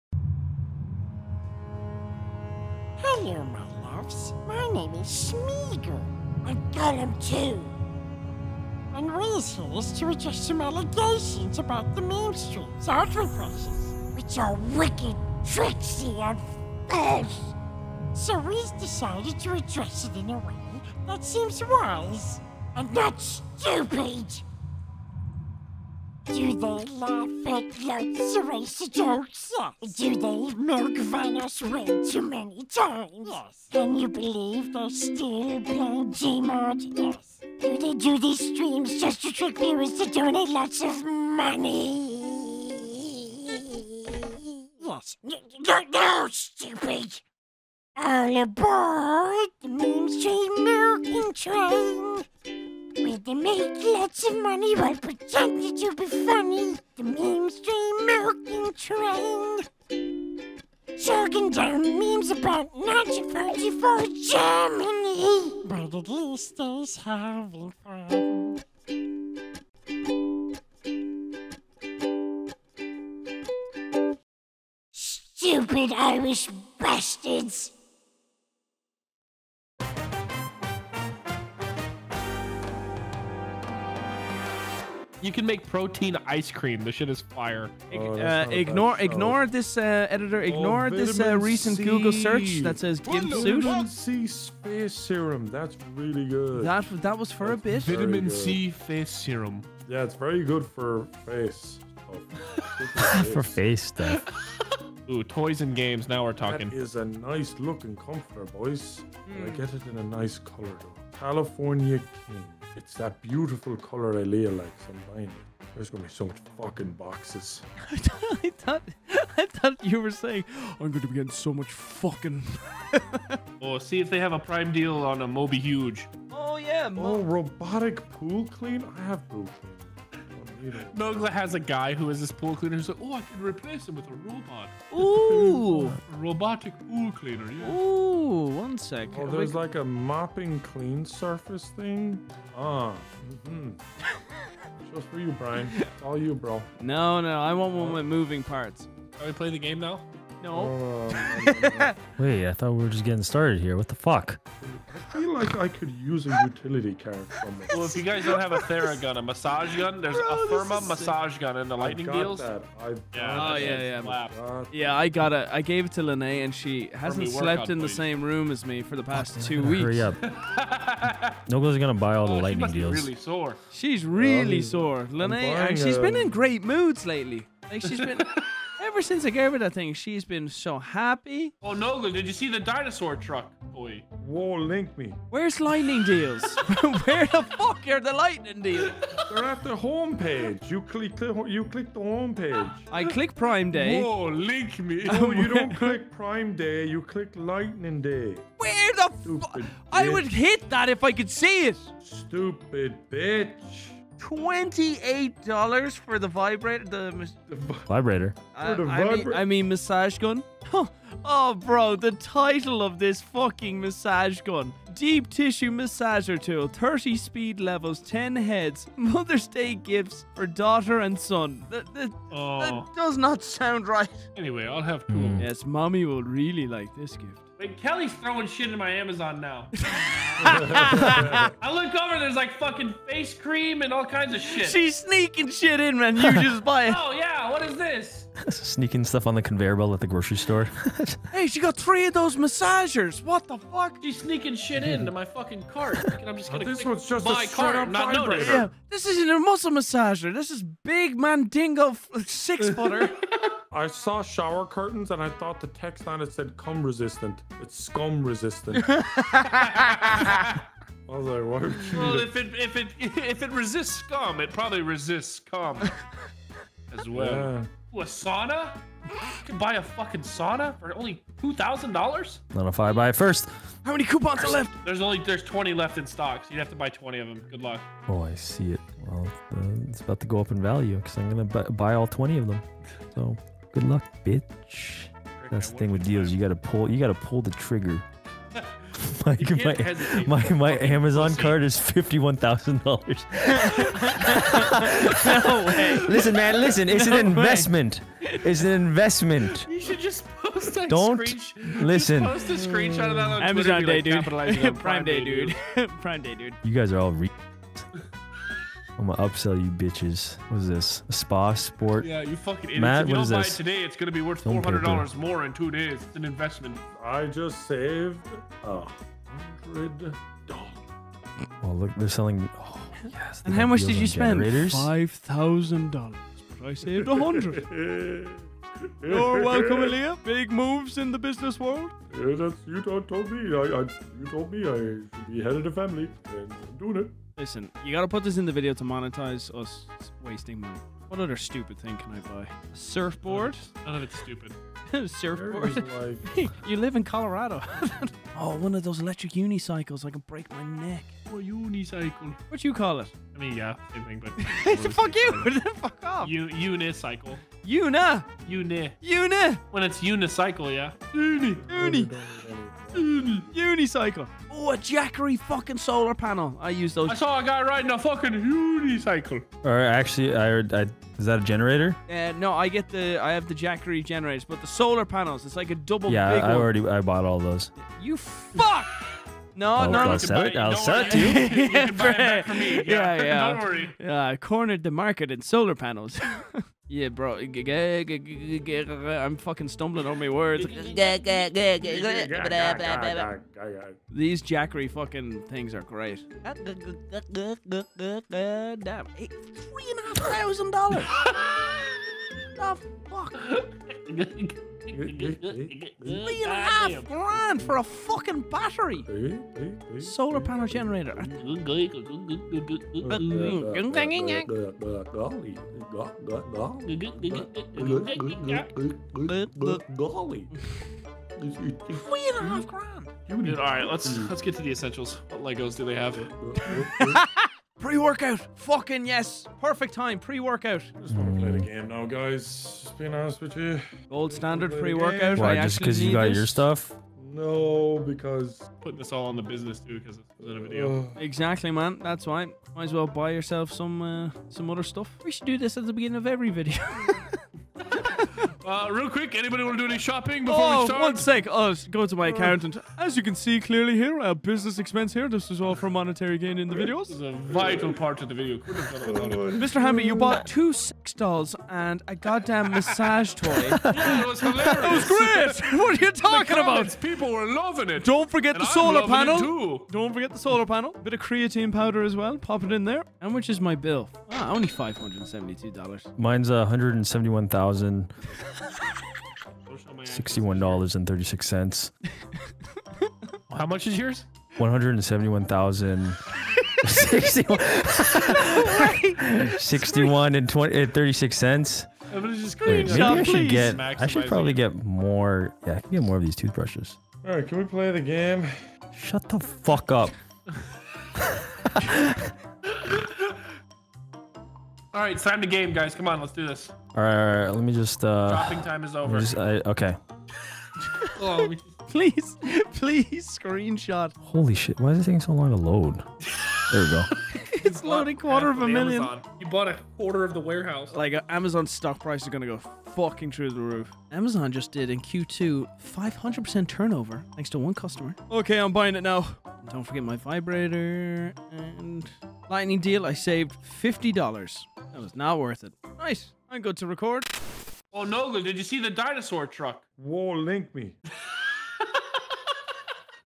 AMAZON PRIME DAY shopping before we start recording! 💸💸💸
Before we started recording a video, we got distracted by shopping for gifts on Amazon Prime Day! We'll be finding some deals on Solar Panels, Hot Tubs and some stuff I had to blur!

TERRORISER
19215 SE 34TH ST
STE 106 BOX 188
CAMAS, WA 98607